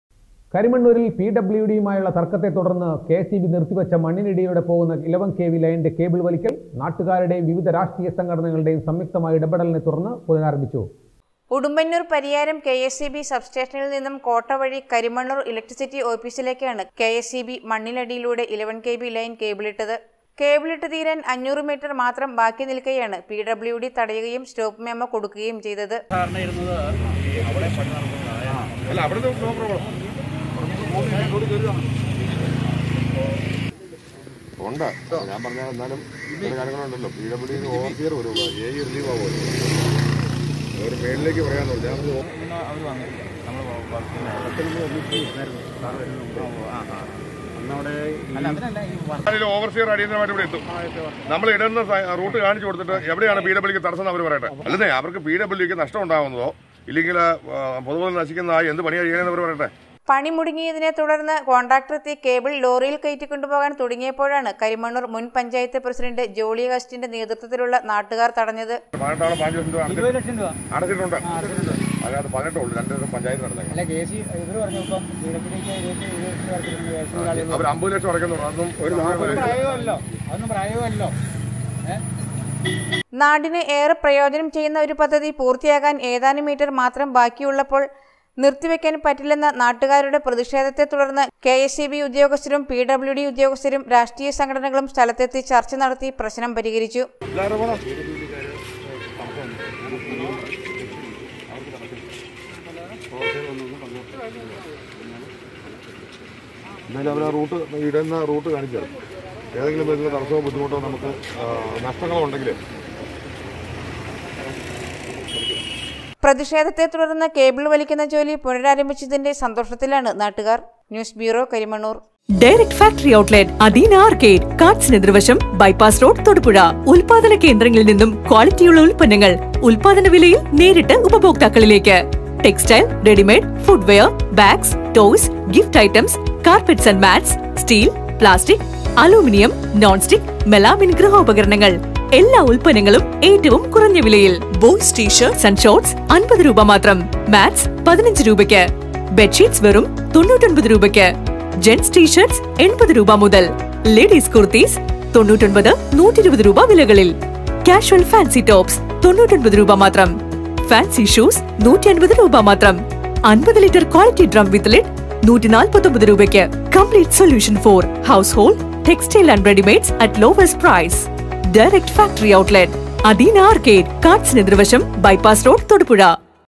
ഇലവന്‍കെവി ലൈന്‍ വലിക്കല്‍ നാട്ടുകാര്‍ ഇടപെട്ട് പുനരാരംഭിച്ചു
കരിമണ്ണൂരില്‍ പി ഡബ്യൂ ഡി യുമായുള്ള തര്‍ക്കത്തെ തുടര്‍ന്ന് കെ എസ് ഇ ബി നിര്‍ത്തി വച്ച മണ്ണിനടിയിലൂടെ പോകുന്ന  ഇലവന്‍ കെ വി ലൈനിന്റെ കേബിള്‍ വലിക്കല്‍  നാട്ടുകാരുടെയും വിവിധ രാഷ്ട്രീയ സംഘടനകളുടേയും സംയുക്തമായ ഇടപെടലിനെ തുടര്‍ന്ന് പുനരാരംഭിച്ചു.